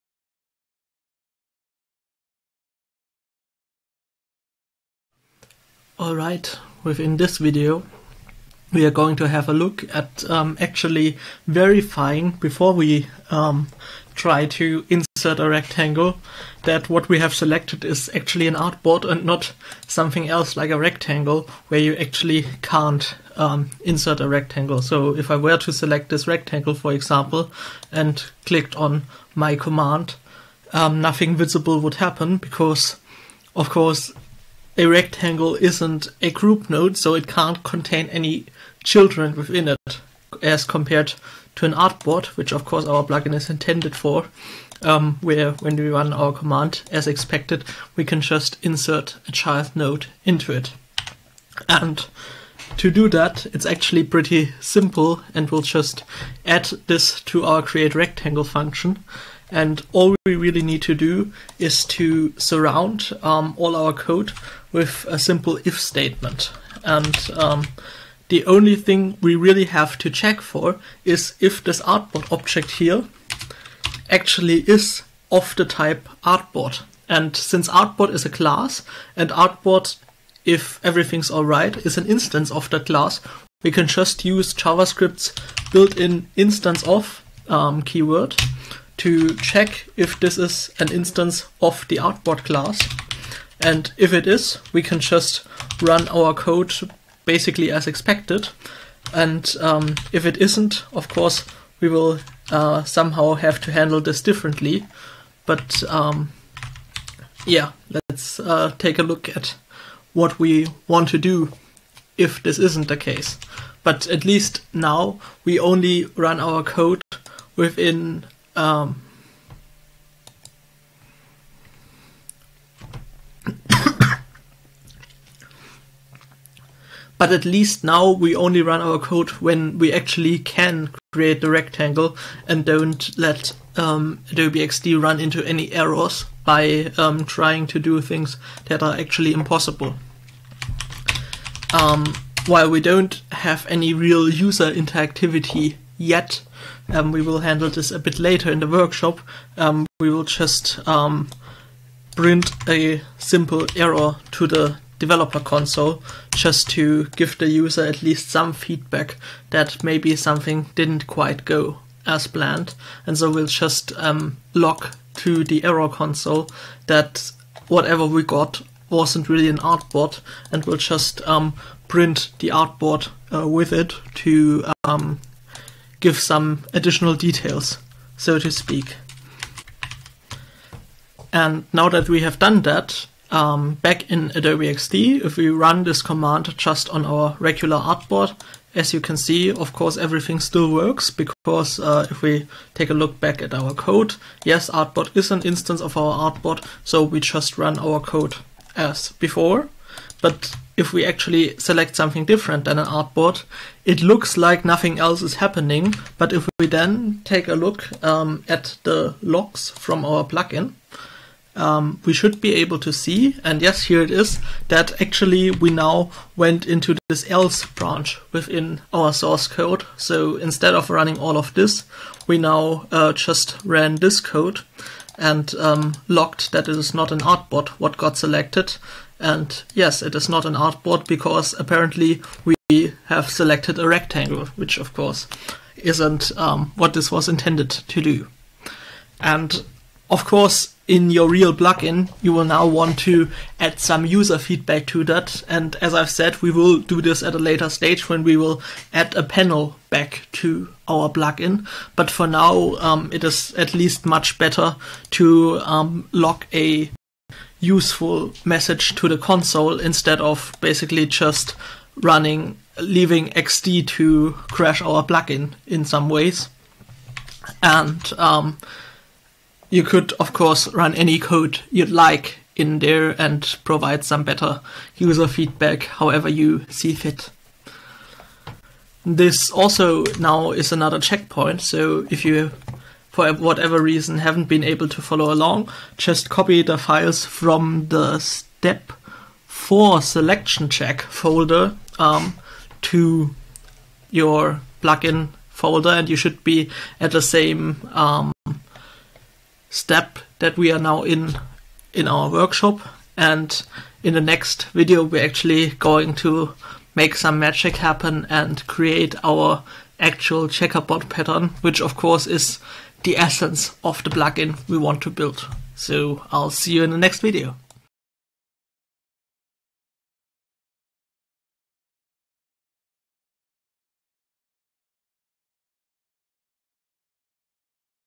Developing an XD Plugin #4 | Verifying artboard is actually an Artboard
Learn how to develop plugins for Adobe XD using the unified extensibility platform (UXP).

Pablo Klaschka, the developer of the Lorem Ipsum plugin for Adobe XD, introduces you to all the important concepts you need to know to develop your first own plugin. While basic JavaScript knowledge is recommended, it's not strictly required.